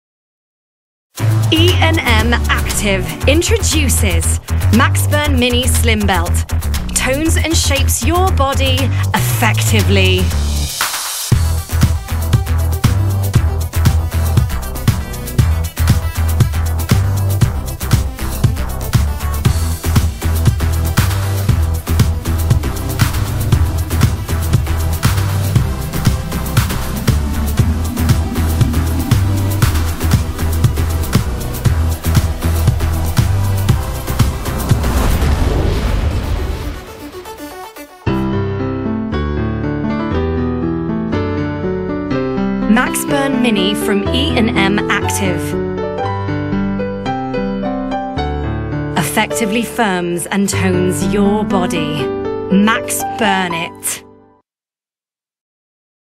MAXBURN MINI slim belt ~ The Film
MAXBURN MINI from E&M ACTIVE
Effectively firms and tones your body
Maxburn it!

Please consult your physician before embarking on any new exercise routine.